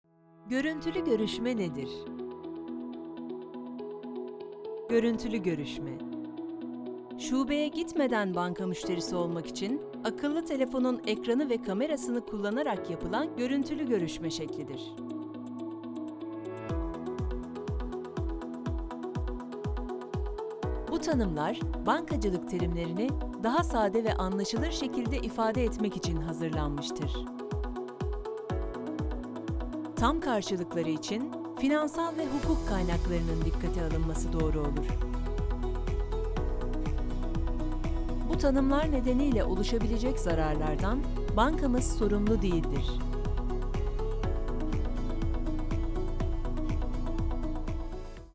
Görüntülü Görüşme Nedir? | Alternatif Bank
Görüntülü Görüşme Nedir? Şubeye gitmeden banka müşterisi olmak için akıllı telefonun, ekranı ve kamerasını kullanarak yapılan görüntülü görüşme şeklidir.

Şubeye gitmeden banka müşterisi olmak için akıllı telefonun, ekranı ve kamerasını kullanarak yapılan görüntülü görüşme şeklidir.